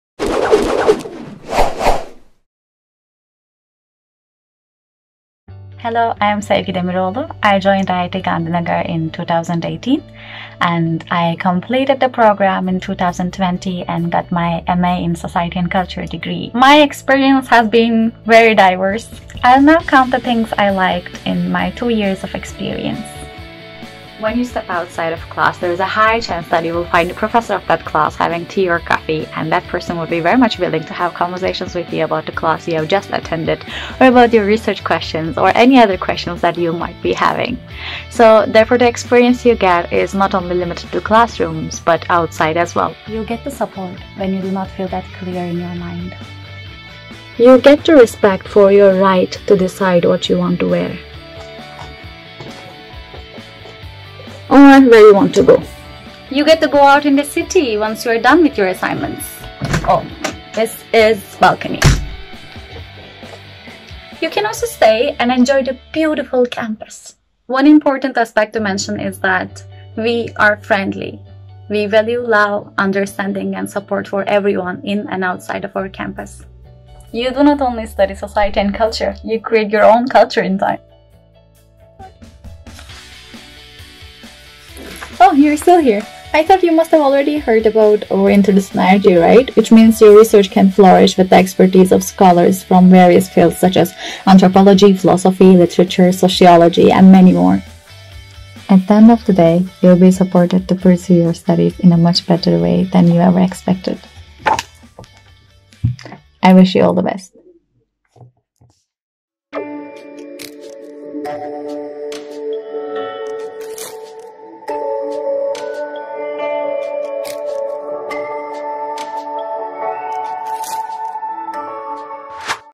IITGN Alum Speaks | Ft. Sevgi Demiroglu
Sevgi Demiroglu is a fresh 2020 graduate from the MA in Society and Culture programme offered by IIT Gandhinagar. In this video, she talks about the diverse experiences gained during her two years of stay at the beautiful IITGN campus. The supportive and open-minded community at IITGN gave her the space to express herself freely. Along with academic and research-based support from the multidisciplinary faculty, Sevgi highly appreciates the discussions and learning opportunities from outside the classroom.